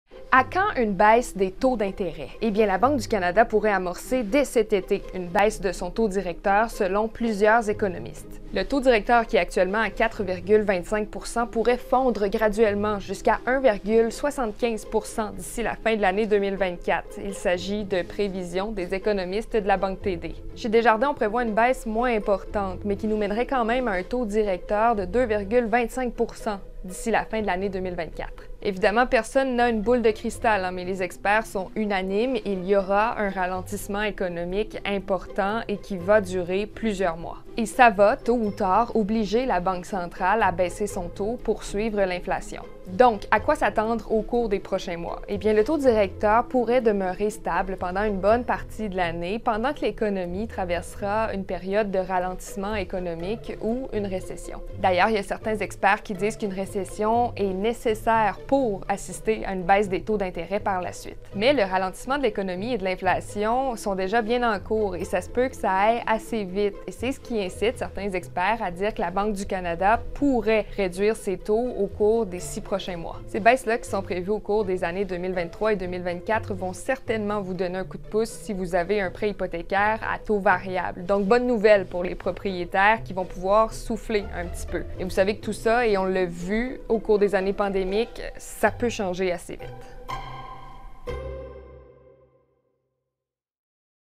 QUESTION DU PUBLIC | À quand une baisse des taux d'intérêts?
Alexane répond à cette question du public.